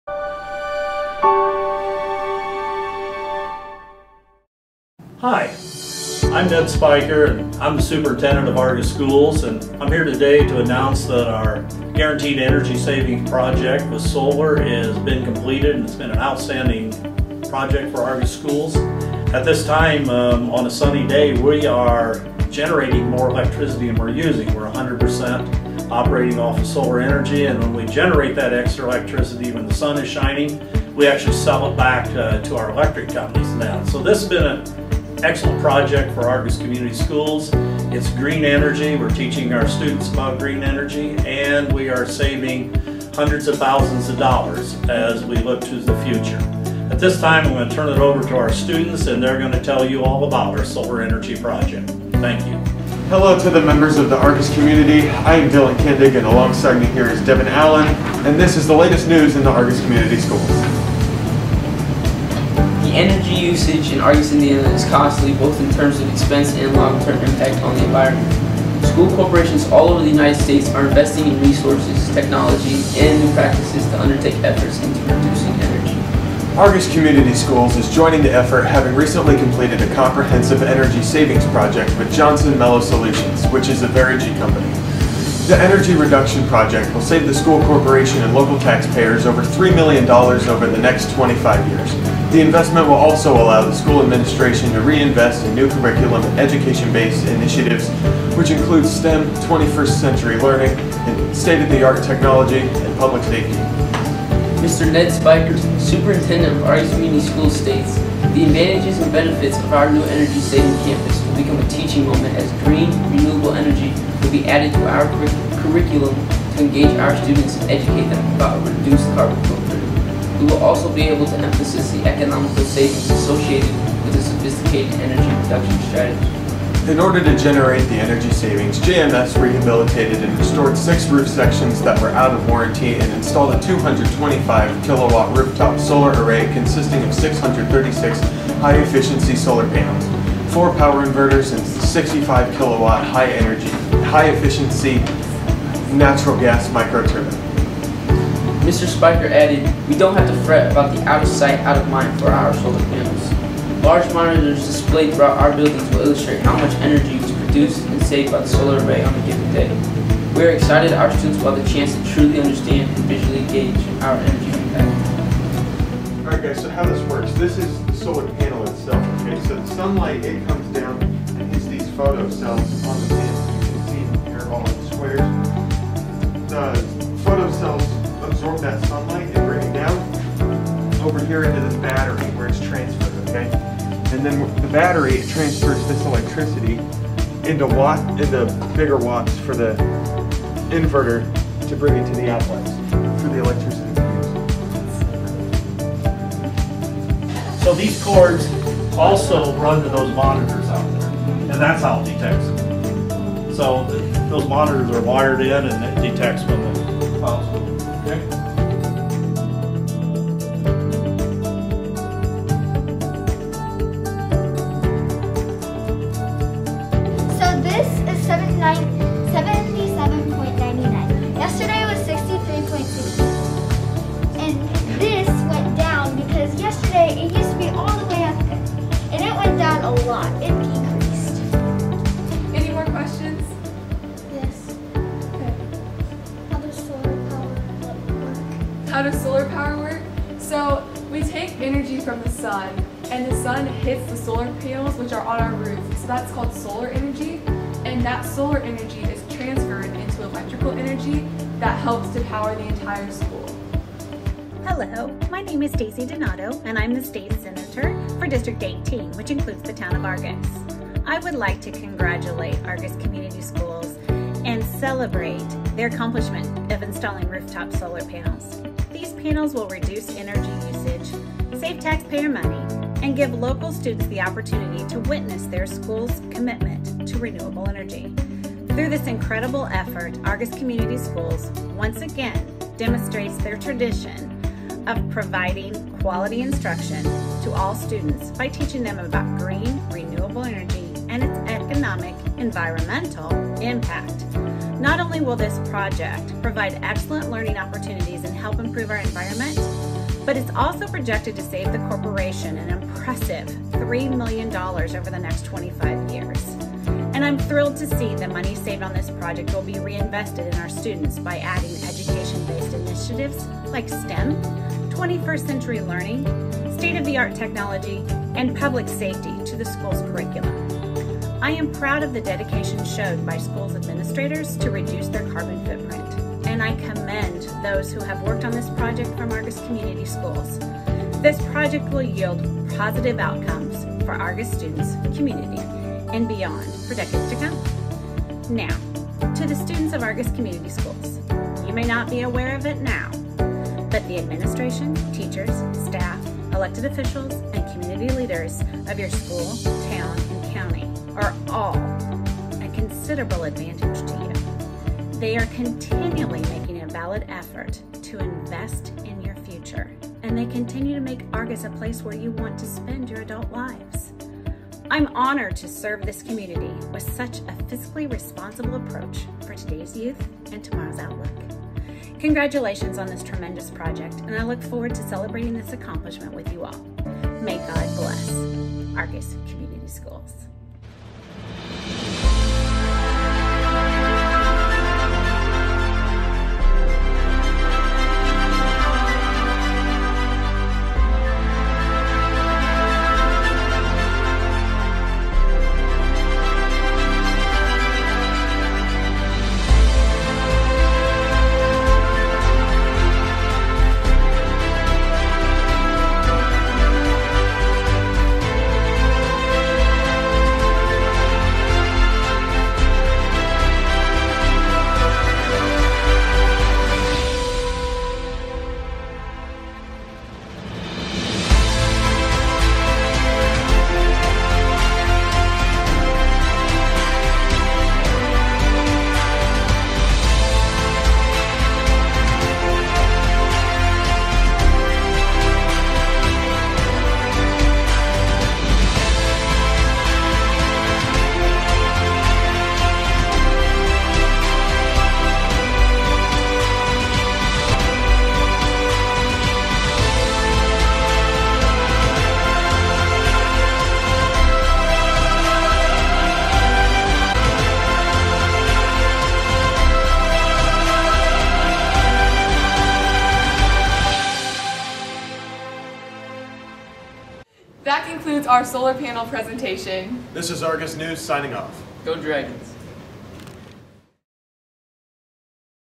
Argos Community Schools Solar Project
Argos students produce and star in a video about the recent Solar Project.